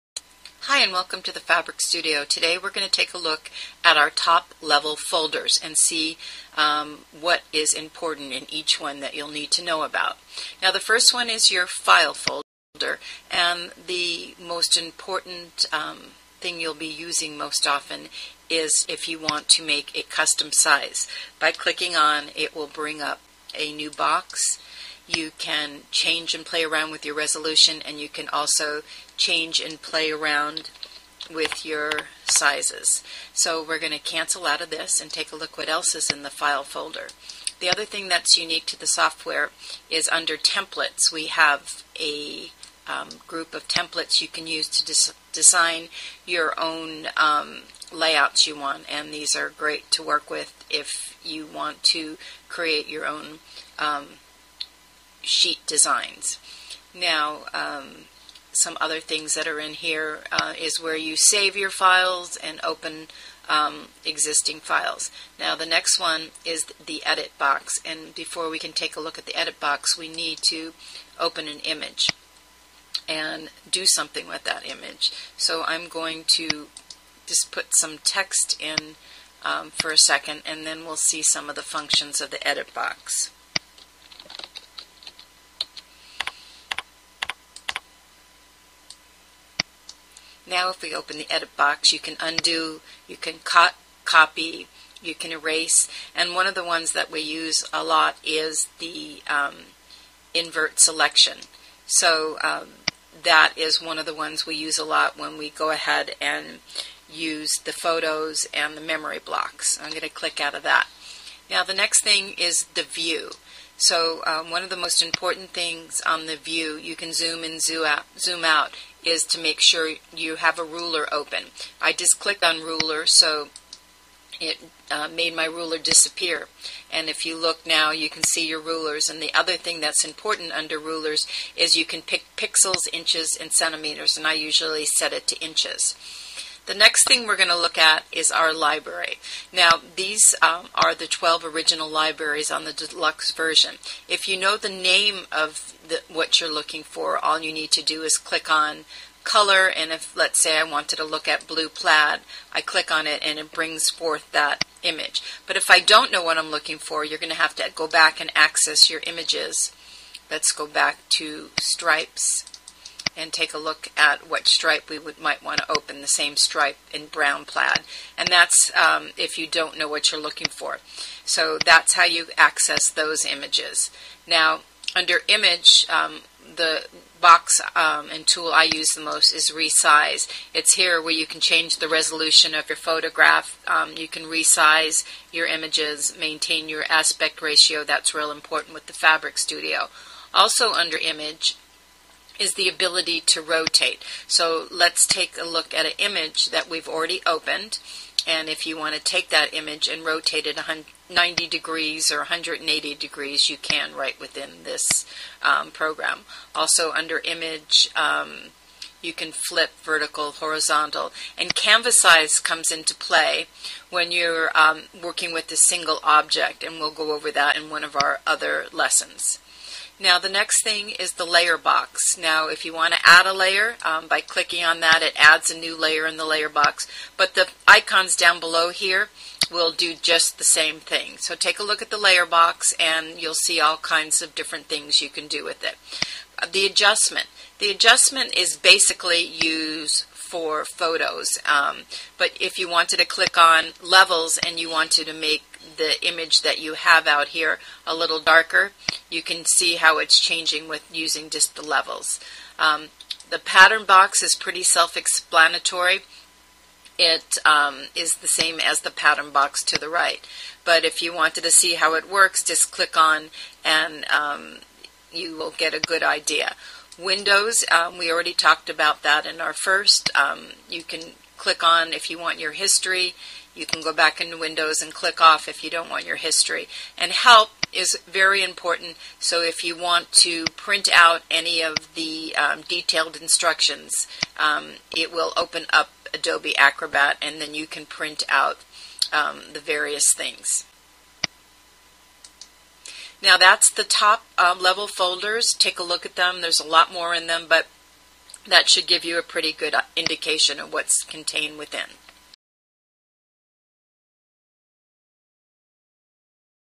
Fabric Studio Software-a look at the Top Level Folders within.
Lets take a look at the Top Level Folders within the Fabric Studio Software.  Check out each folder before starting your designs.